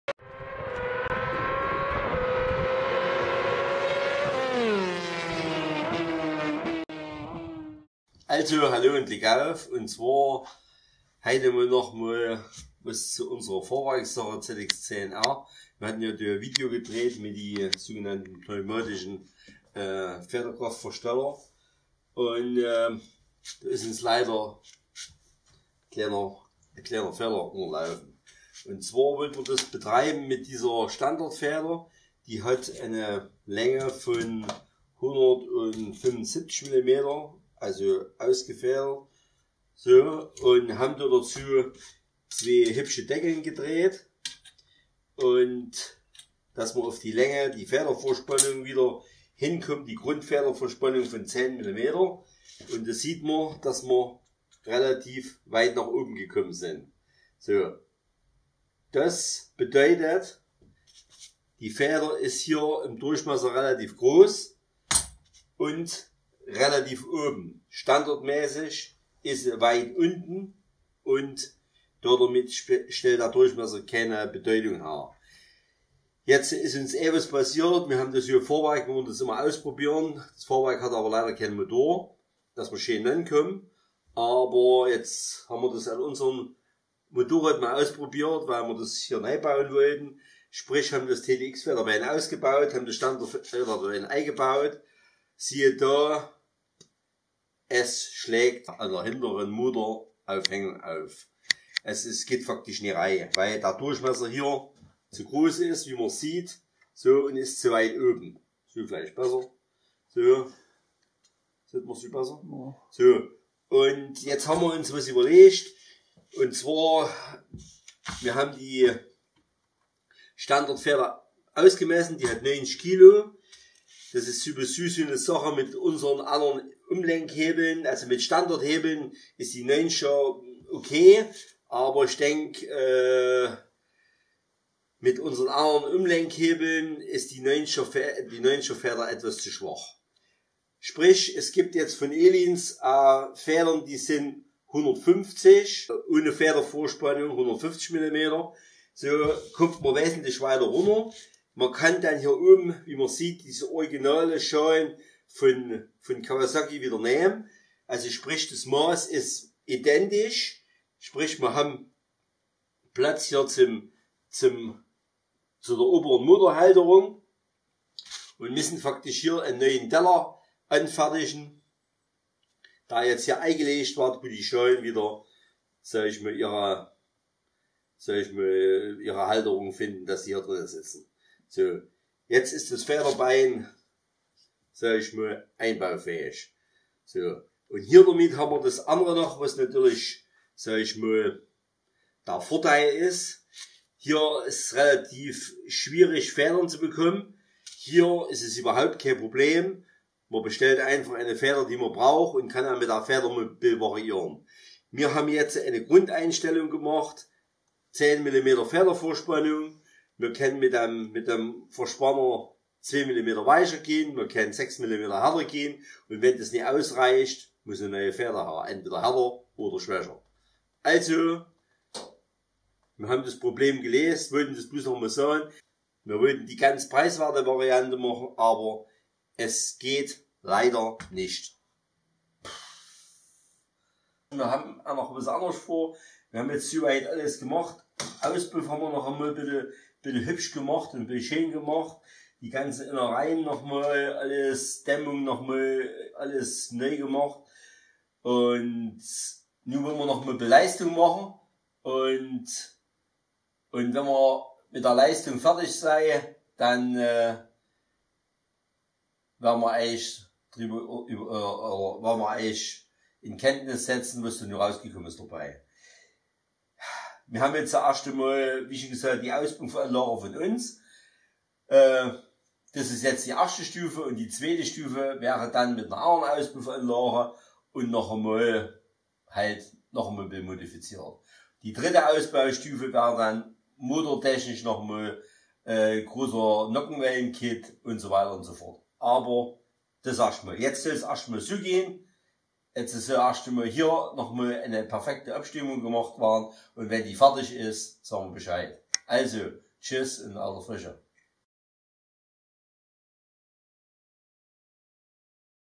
Kawasaki ZX10R Update pneumatischer Versteller Federbein Showa / Öhlins
Hier noch eine Korrektur zu unserem pneumatischen Vorspannungsversteller...
Die Versteller passen zwar mit der originalen Showa Feder auf das Showa Federbein, allerdings haben wir den Einbau ohne Motor getestet. Die Feder sitzt hier leider zu weit oben und kommt an die Motoraufhängung. Man kann also das originale Federbein fahren, muss dann aber eine kurze z.B. Öhlins Feder verwenden.

Als nächstes gehts auf den Prüfstand...

Bei Interesse oder Anregungen einfach melden.